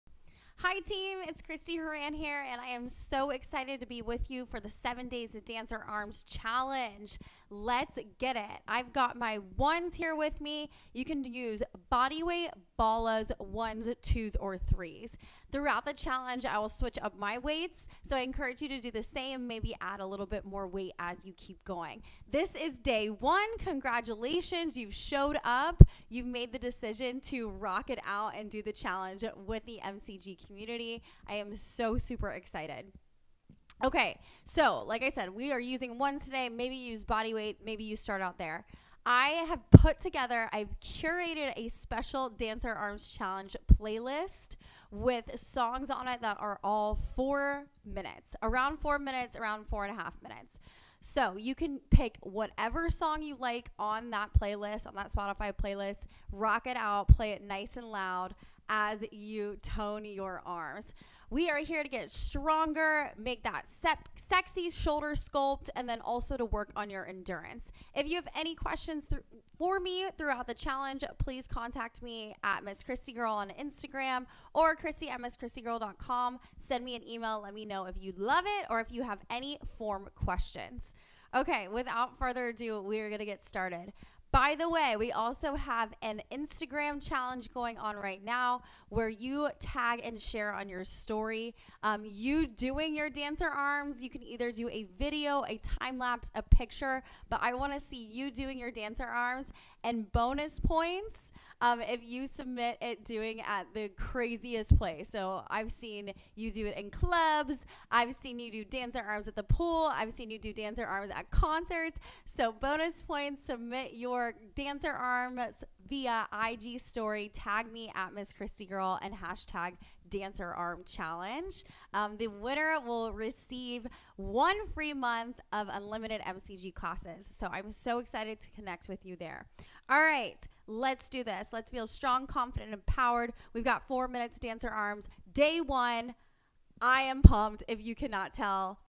Intro to Dancer Arms
Congratulations! You're officially signed up for the 7 Days to Dancer Arms Challenge.
Only 4 minutes per day for 7 days. Grab your dancer arms weights (3lb/ 2lb/ 1lb/ Balas/ bodyweight) and blast your favorite upbeat song! Your goal is to stay consistent for 7 days.

Win a FREE month of MCG livestream and on-demand classes!
​Participate in the Dancer Arms Instagram Challenge. Check in with me on IG Stories every day of the 7 Days to Dancer Arms Challenge between today (7/4) and Monday, 7/18. Post your sweaty selfie, take a time lapse of your dancer arms, or post of a picture of you doing dancer arms in the CRAZIEST location to your Instagram stories and make sure you tag @misschristiegirl and DancerArmsChallenge.